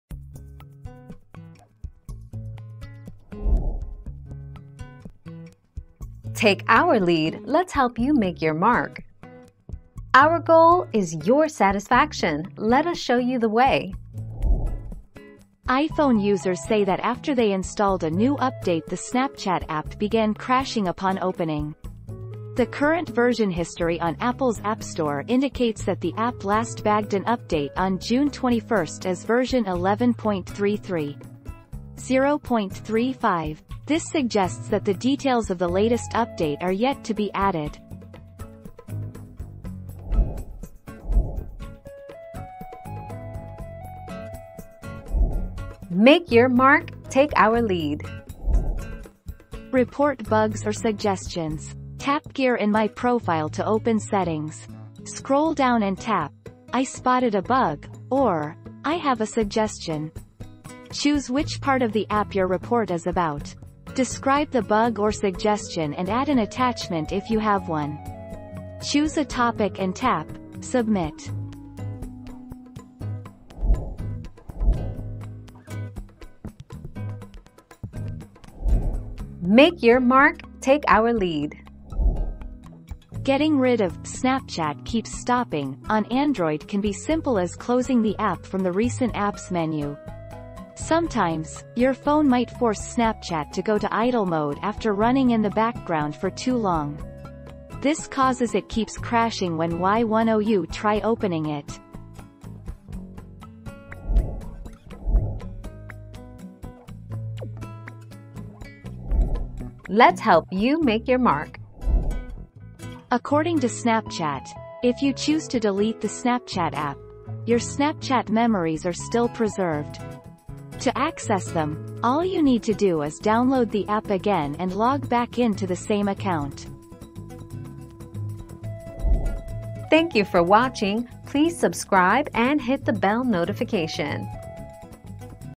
Why does Snapchat crash on my iPhone?
00:00 - Why does Snapchat crash on my iPhone?
00:42 - Where do I report Snapchat problems?
01:15 - Why is snap kicking me out of the app?
01:45 - Does deleting Snapchat delete memories?

Laura S. Harris (2021, August 11.) Why does Snapchat crash on my iPhone?
    AskAbout.video/articles/Why-does-Snapchat-crash-on-my-iPhone-259886